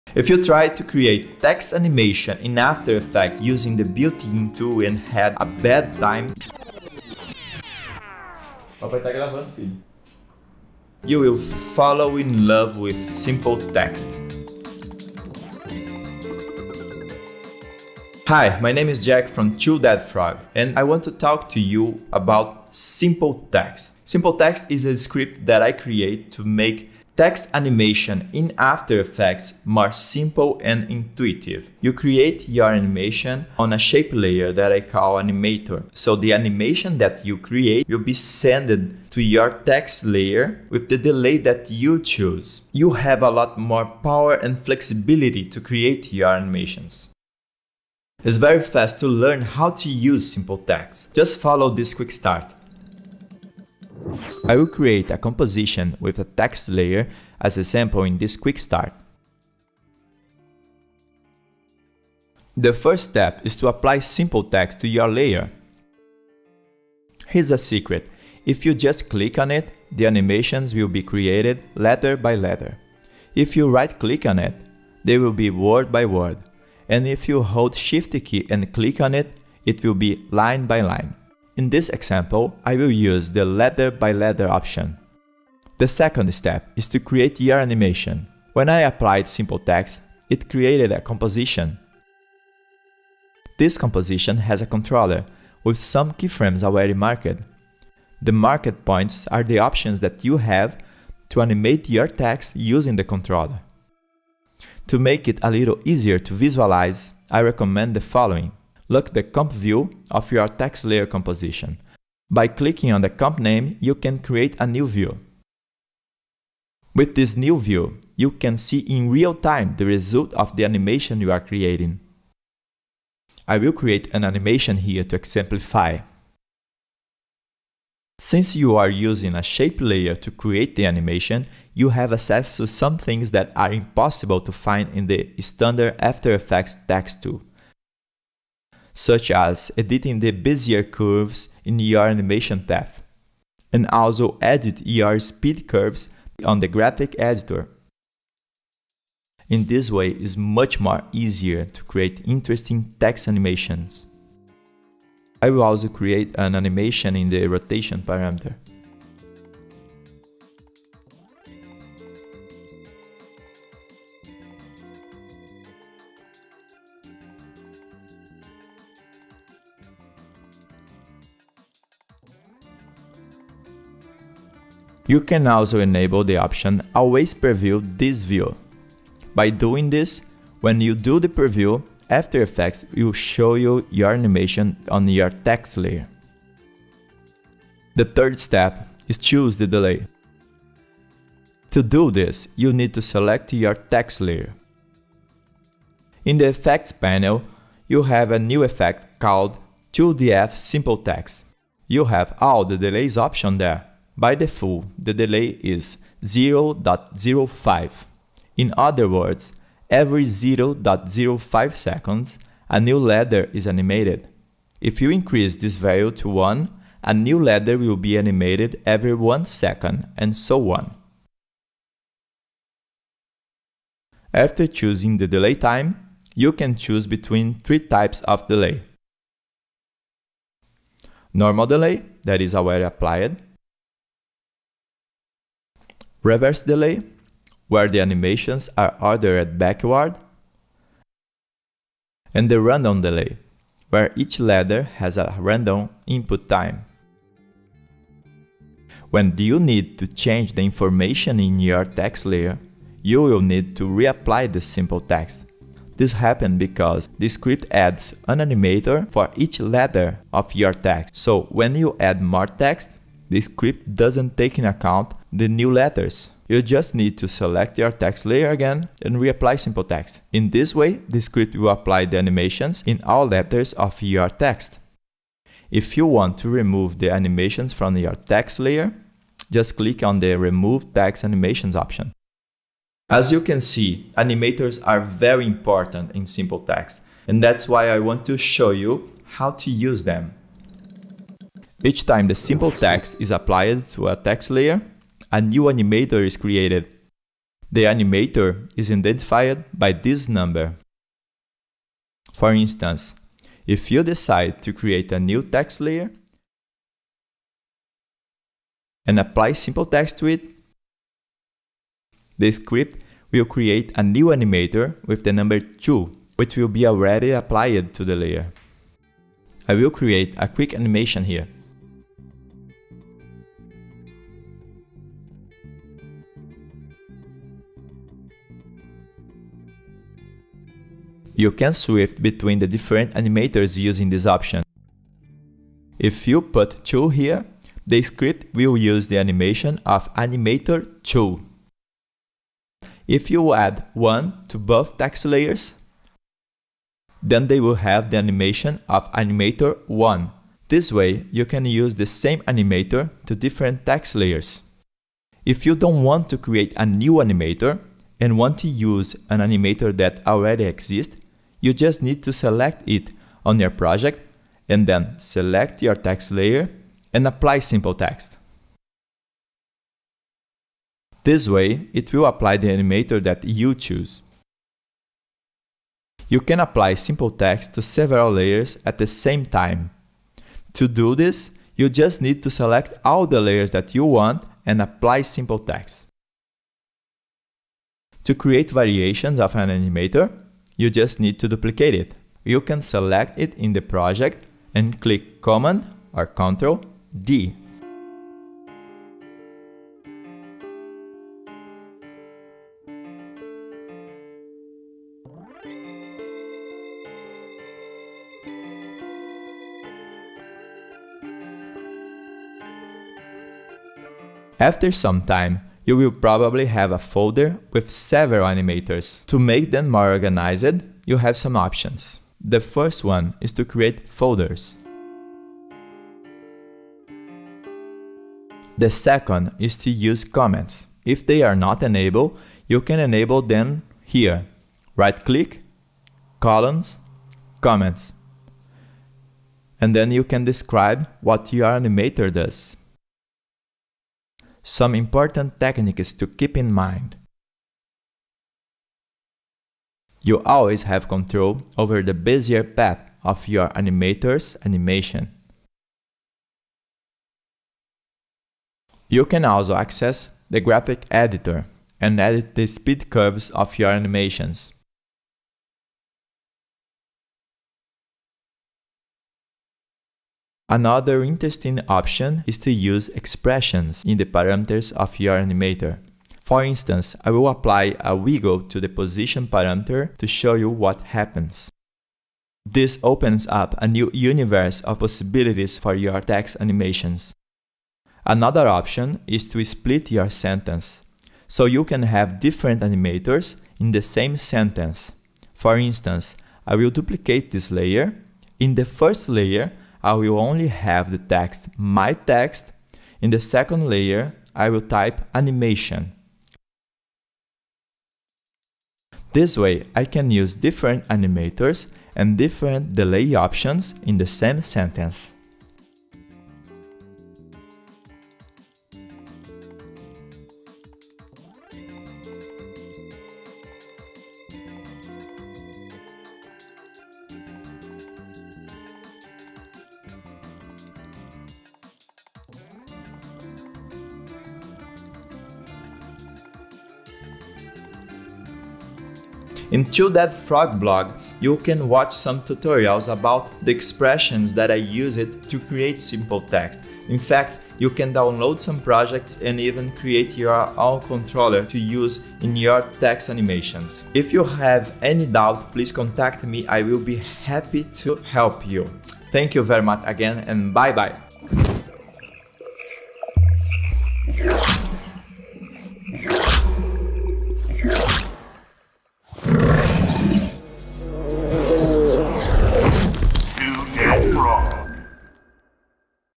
Simple Text: Text Animation Script for After Effects Quick Start
Quick start of Simple Text.

Simple Text is an After Effects script that  simplifies text animation making it faster and intuitive.

It uses a shape layer to control the text animation. The timing and type of the delay between the letters are easily controlled in just a few clicks. Once created, you can apply the animation to any text layer.

Create text animations in 3 steps:
1. Convert your text layer.
2. Create your animation.
3. Define the type of delay and the time.